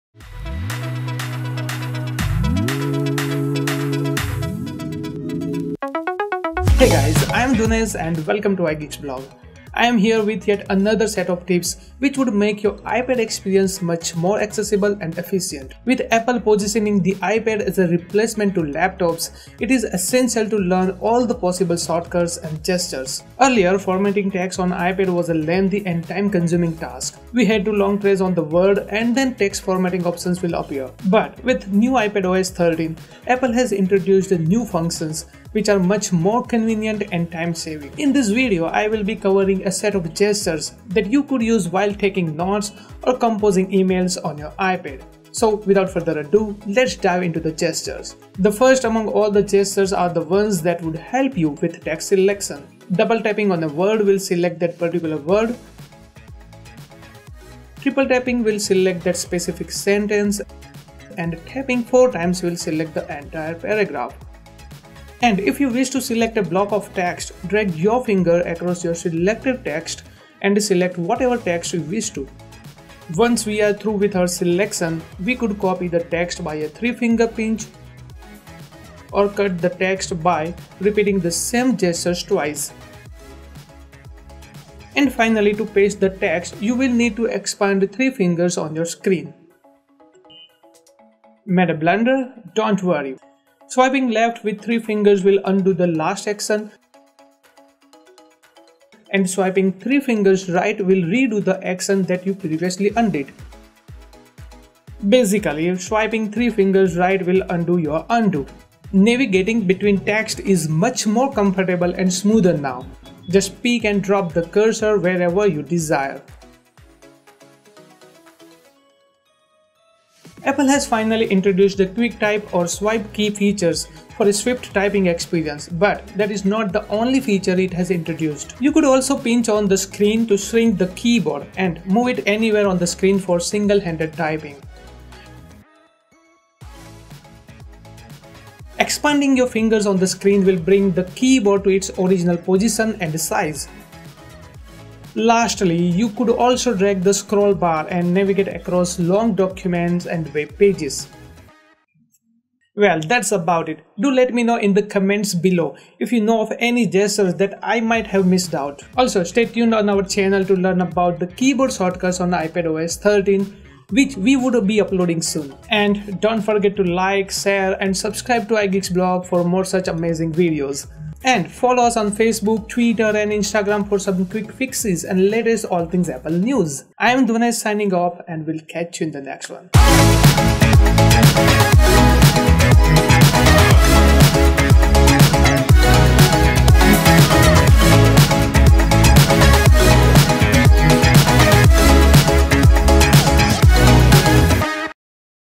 iPadOS 13 - New Text Formatting Gestures in iPad Pro
iOS 13 and iPadOS 13 has introduced a bunch of new features. Let's find out new text editing and formatting gestures on iPad Pro running iPadOS 13.

iPadOS 13 Text Selection and formatting: 00:48
iPadOS 13 Change the way we Navigate and Edit Text: 01:48
iPadOS 13 keyboard hacks: 2:00

Or